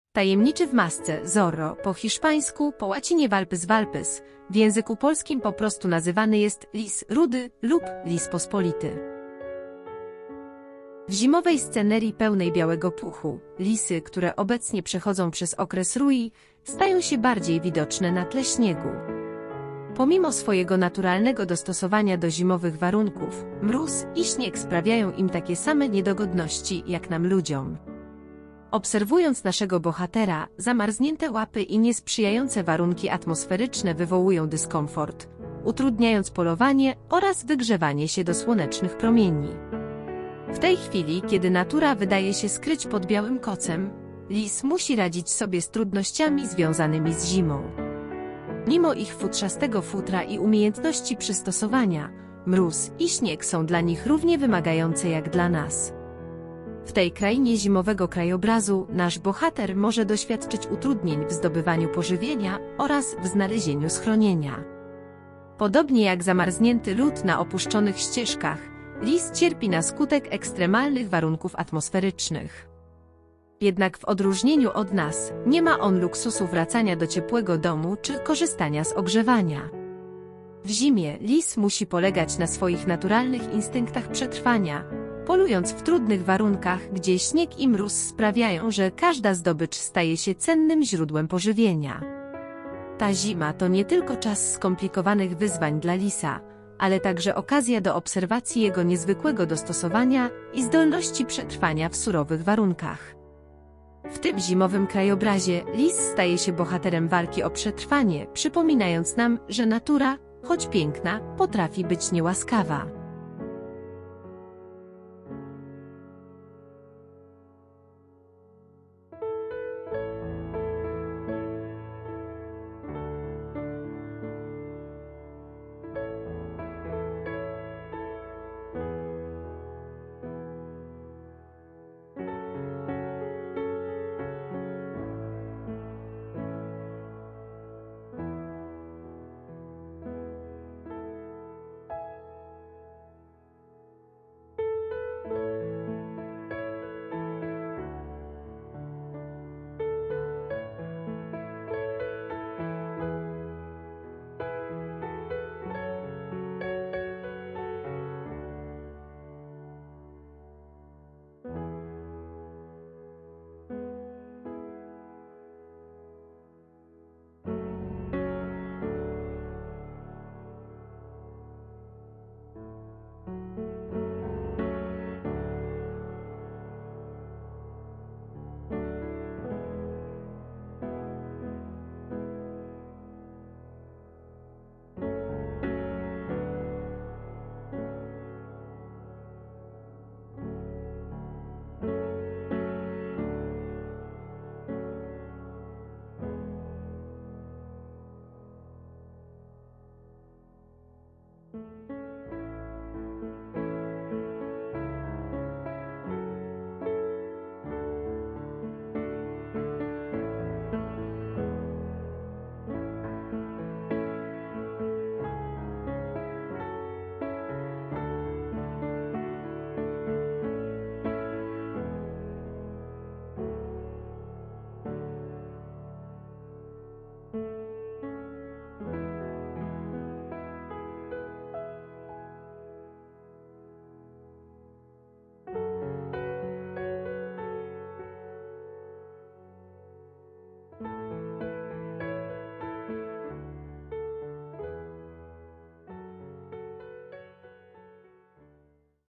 Zamaskowany Spryciarz
Zdolności lisa, takie jak finezja, spryt i odwaga, przeniesione na grunt ludzki, uczyniły postać Zorro niezapomnianym bohaterem. Książki i filmy opowiadające o jego nieustraszonych przygodach zrobiły furorę, zdobywając serca widzów na całym świecie. Legendarny Zorro stał się ikoną sprawiedliwości, inspirującą kolejne pokolenia do walki przeciwko ciemności i niesprawiedliwości.